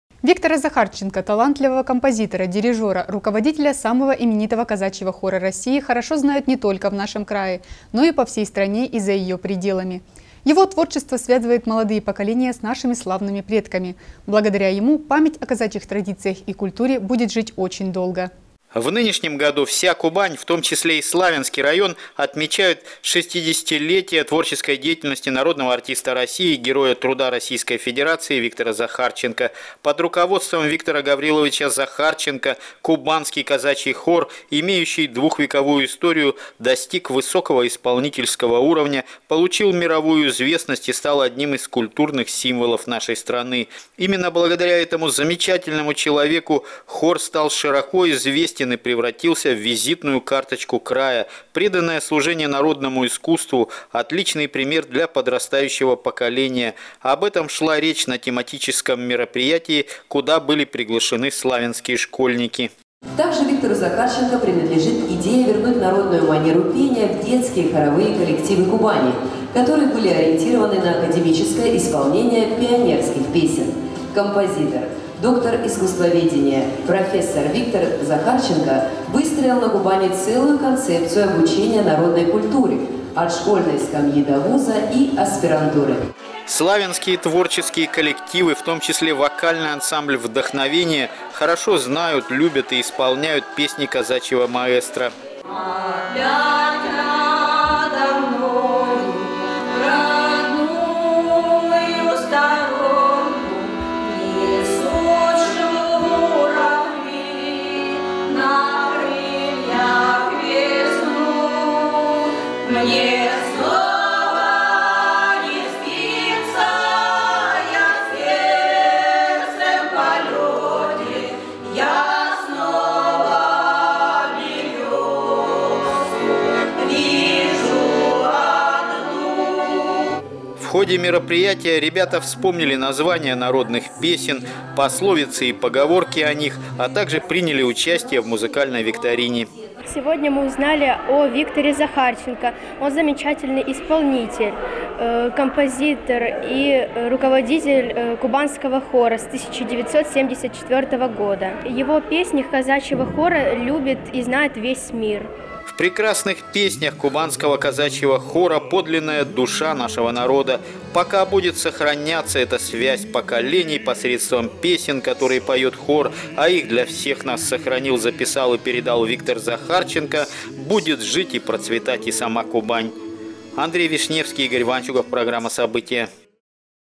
Творчество, посвященное родной Кубани
Виктора Захарченко — талантливого композитора, дирижера, руководителя  самого именитого казачьего хора России хорошо знают не только в нашем крае, но и по всей стране и за ее пределами. Его творчество связывает молодые поколения с нашими славными предками. Благодаря ему  память о казачьих традициях и культуре будет жить  очень долго